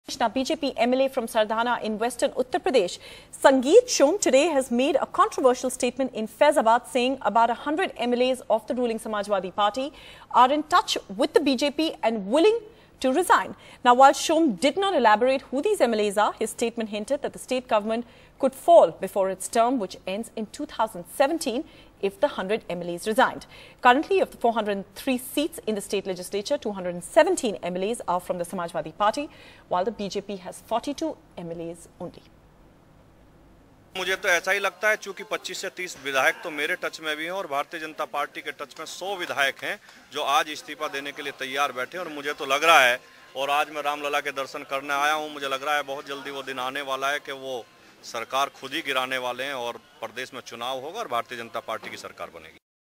Over 100 Samajwadi Party legislators ready to join BJP, says lawmaker Sangeet Som
Sangeet Som, the BJP lawmaker from Sardhana who had been arrested in the Muzaffarnagar riots case, created a controversy today, saying around a hundred lawmakers from the ruling Samajwadi Party are in touch with the BJP, willing to bring down the Akhilesh Yadav government.